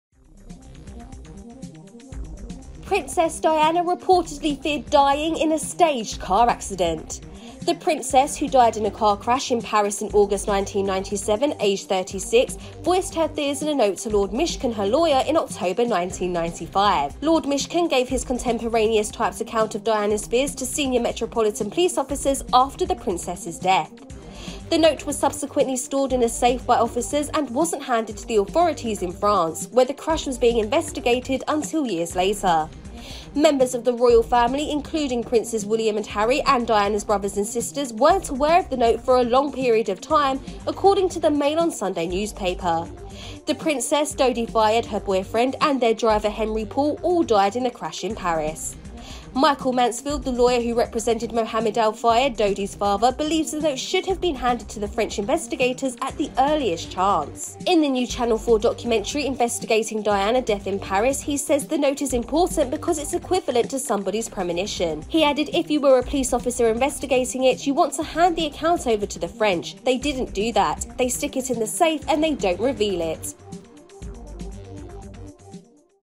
Princess Diana 'feared dying in a staged accident'
The princess - who died in a car crash in Paris in August 1997, aged 36 - voiced her fears in a note to Lord Mishcon, her lawyer, in October 1995.
Lord Mishcon gave his contemporaneous typed account of Diana's fears to senior Metropolitan Police officers after the princess' death.
The note was subsequently stored in a safe by the officers and wasn't handed to the authorities in France - where the crash was being investigated - until years later.
Members of the royal family - including Princes William and Harry, and Diana's brother and sisters - weren't aware of the note for a long period of time, according to the Mail on Sunday newspaper.
The princess, Dodi Fayed - her boyfriend - and their driver, Henri Paul, all died in the crash in Paris.
Michael Mansfield - the lawyer who represented Mohamed Al Fayed, Dodi's father - believes the note should have been handed to the French investigators at the earliest chance.
In the new Channel 4 documentary 'Investigating Diana: Death In Paris', he says: "The note is important because it's equivalent to somebody's premonition.” He added: "If you were a police officer investigating it, you want to hand the account over to the French. They didn't do that. They stick it in the safe and they don't reveal it."